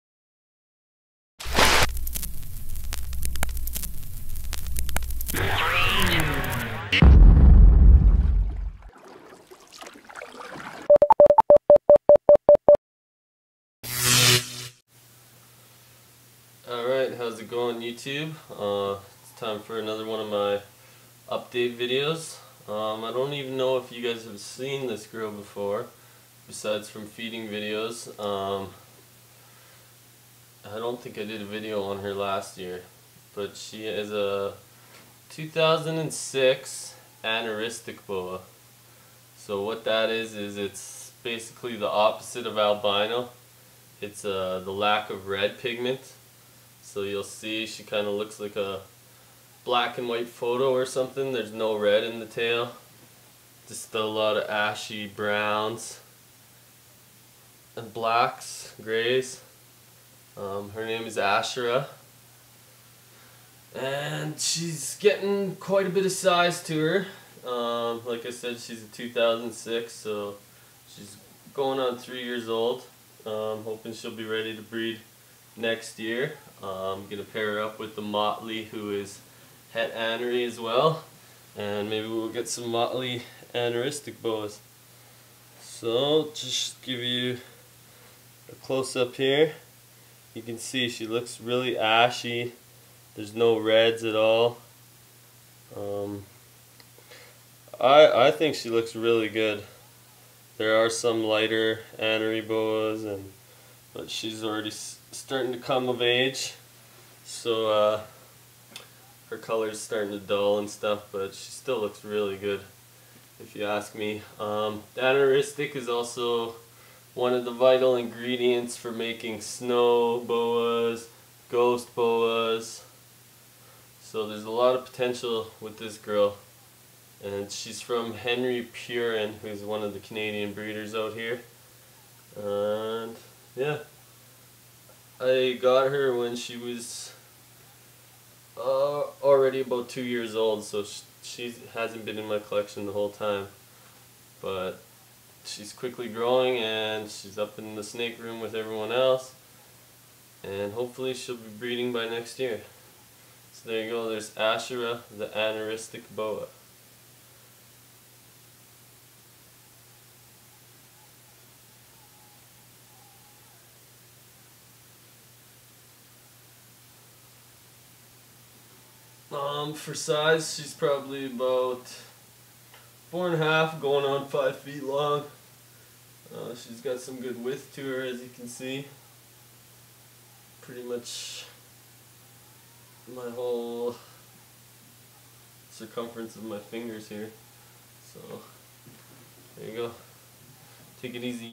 Asherah 2008
Pre- recorded update video on Asherah my 2006 Anery Boa constrictor